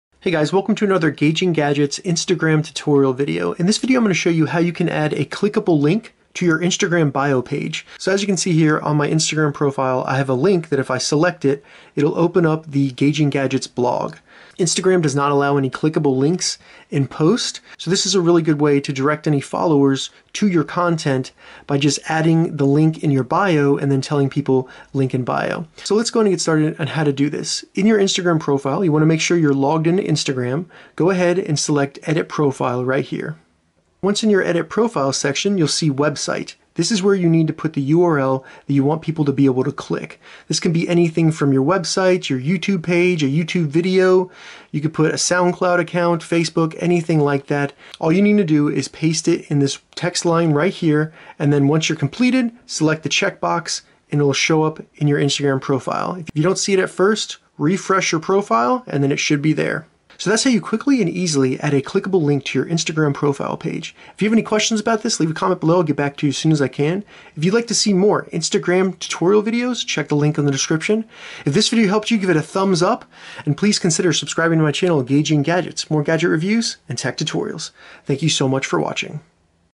Add a Clickable Link in Your Instagram Bio - Instagram Tutorial
How to add a link to your Instagram profile? In this tutorial, I show you how to easily share links on Instagram.  This makes it really easy to share your latest content with your users. This includes blogs, websites, YouTube Channels, Twitter handles, Twitch Channels, Streaming Platforms, vlogs, SnapChat, Facebook and other social media links.

Adding clickable links to an Instagram post is currently impossible. The only way to share websites or links with your followers is to add a link to your bio and then direct your followers to your Instagram profile for the link.

Follow the steps below to add a clickable link to your Instagram bio:
1. Login to Instagram and go to your profile page.
2. Select Edit Profile.
3. Find the text field labeled Website and add the link to that field.
4. To save your Instagram profile, select the check mark in the top right of the app.
5. You have now added a clickable link to your Instagram profile page!